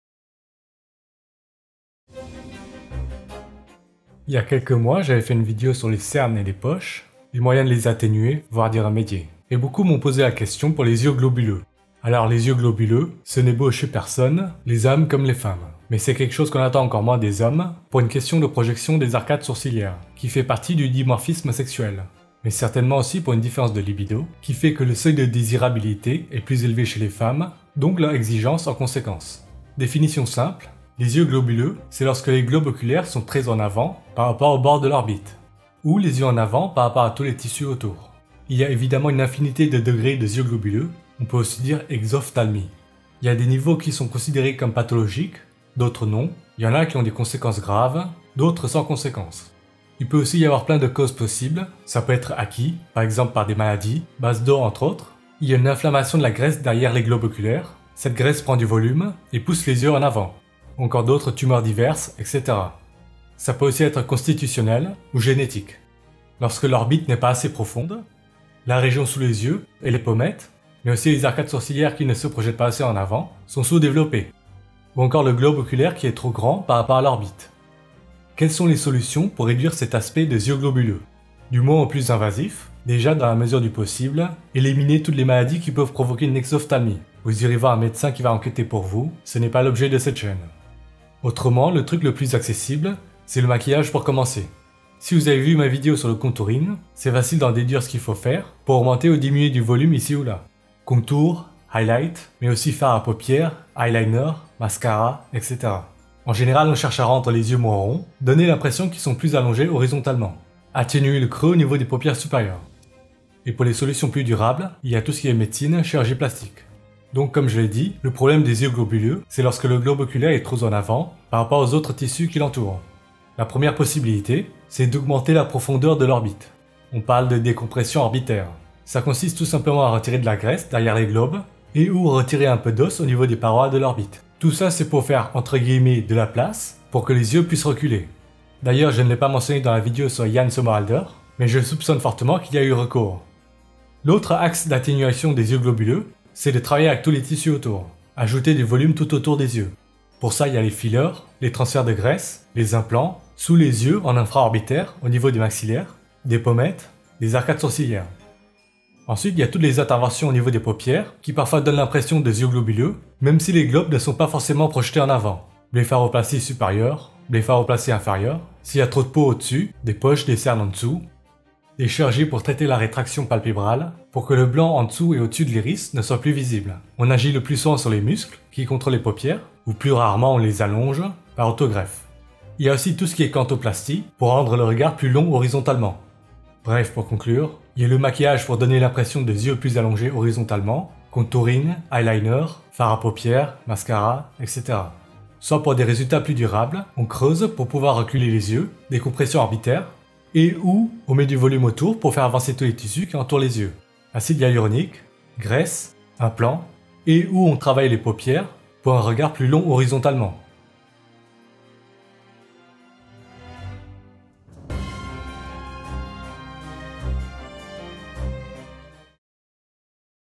YEUX GLOBULEUX, LES SOLUTIONS pour avoir un BEAU REGARD, des yeux de beaux gosses !!
Avoir des yeux globuleux  = lorsque les yeux sont en avant par rapport aux bords de l'orbite, en avant par rapport aux tissus entourant les yeux (maxillaires, pommettes, arcades sourcilières...)
Exemples : Ozil Mesut, Mila Kunis.

Les yeux globuleux, ou exophtalmie, peuvent avoir plusieurs causes :
maladies (basedow entres autres), tumeurs...

être constitutionnels, avoir une origine génétique :
orbites pas assez profondes, maxillaires, pommettes, arcades sourcilières sous-développées...

Les différentes solutions pour corriger des yeux globuleux ou l'aspect d'yeux globuleux sont :
Le maquillage (makeup) par contouring, fard à paupières, eyeliners, mascaras, etc... pour rendre les yeux plus longs horizontalement, diminuer la visibilité des paupières supérieures.

La médecine / chirurgie esthétique, plastique :
- Augmenter l'orbite par décompression orbitaire (retrait de graisse et/ou os)
- Ajouter du volume autour des yeux : dermal filler, transfert de graisse, implants :
Au niveau sous orbitaires, devant les maxillaires, au niveau des pommettes, des arcades sourcilières...
- Correction des paupières : blépharoplasties supérieures ou inférieures, chirurgie de rétraction palpébrale (diminuer l'exposition de la sclère, le blanc des yeux), canthoplastie pour des yeux plus allongés horizontalement.